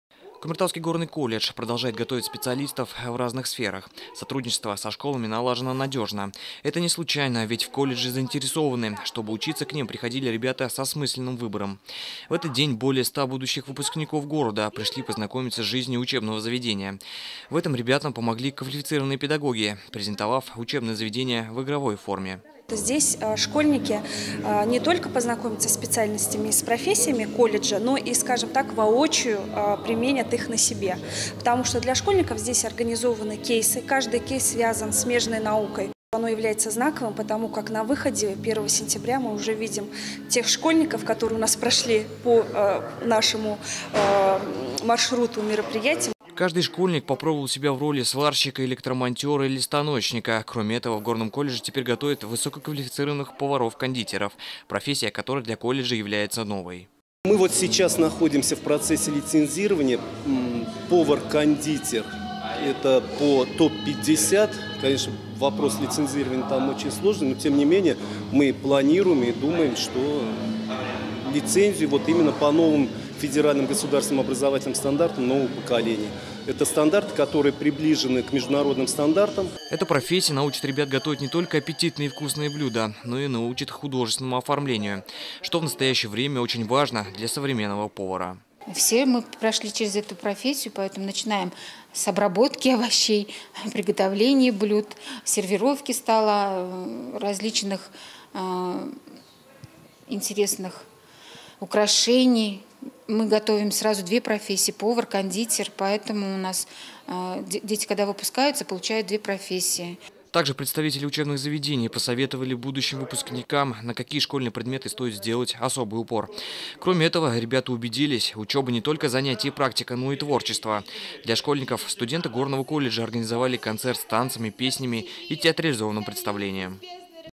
Профориентиация в КГК
Какое учебное заведение выбрать, на какую профессию пойти учиться. Эти вопросы сейчас интересуют почти всех старшеклассников. Так 20 апреля Кумертауский Горный колледж провел профориентационную работу для учащихся 9 и 11 классов. Специалисты вместе с волонтёрами познакомили будущих выпускников с особенностями и принципами разных профессий в нестандартной игровой форме. Подробнее в сюжете Константина Меркушева.